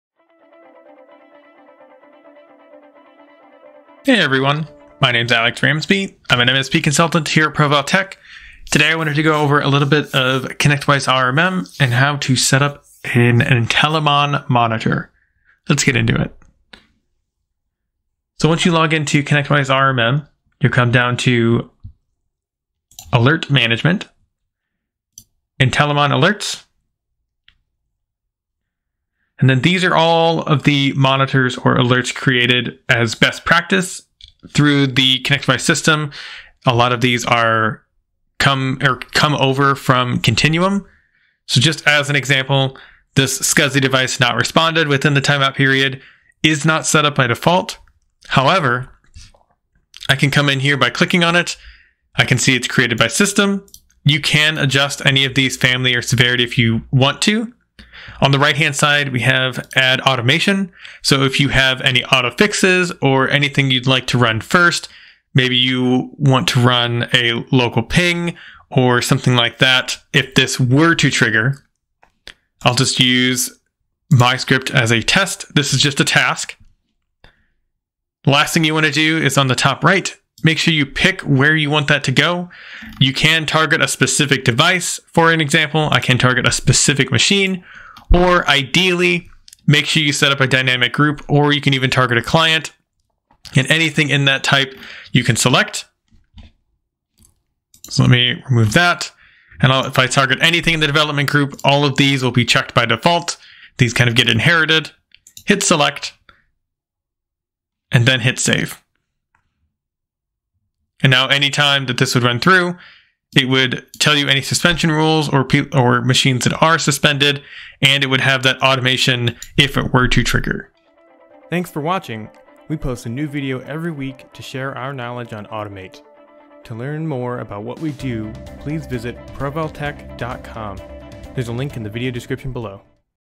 ConnectWise RMM Intellimon Monitors
Happy Tech Tuesday!! This week we are going over the CW RMM Default monitors and how to turn on Automation around them.

ProVal Tech optimizes backend tools for MSPs, if you are interested in learning more check us out